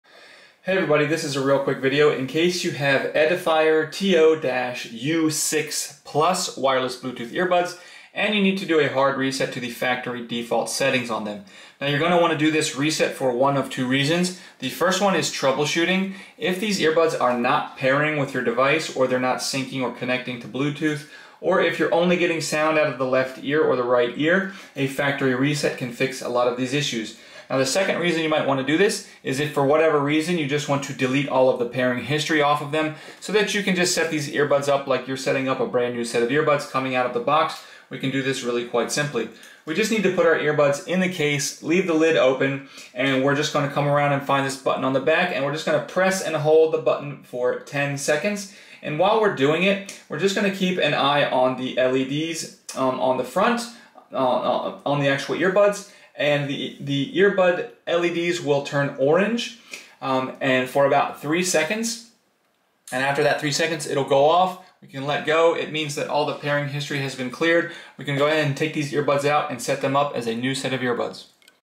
Hard Reset Edifier TO-U6+ Bluetooth Earbuds
How to hard factory reset the bluetooth on Edifier TO-U6+ Earbud earphones if they are not connecting or not pairing or there is only sound out of the left ear or the right ear not working or you need to troubleshoot other problems, a hard reset to factory default settings can fix a lot of issues. Found in the Edifier TO-U6+ Earbuds user manual.

If you need new earbuds, these are some of the highest rated on Amazon